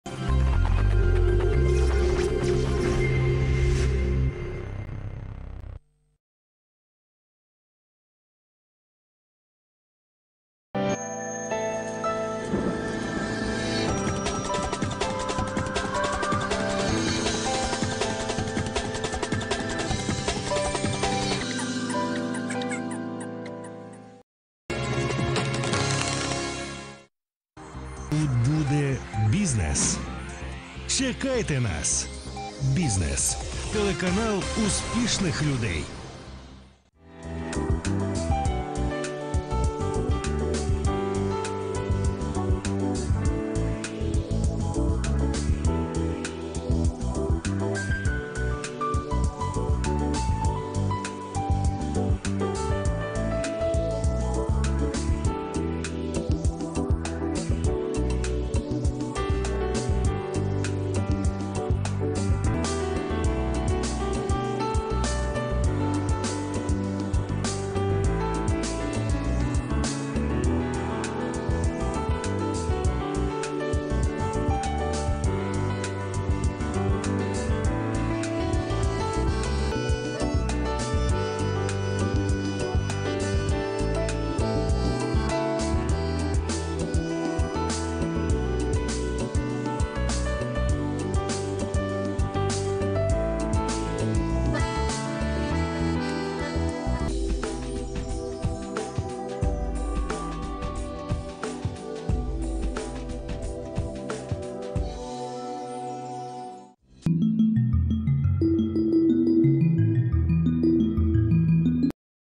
Все заставки канала (Криничівська Погода ТБ, 01.02.2010-04.11.2013)
Сделано по заказу giggles50, firey93.
Большое спасибо †giggles50† / †Константин†, TV&Design (DNTV), Ходаковському, firey93, Serery, Тимуру Неджроби и т.д.
Я не крал видео у пользователей!!! Просто пригодилось!!!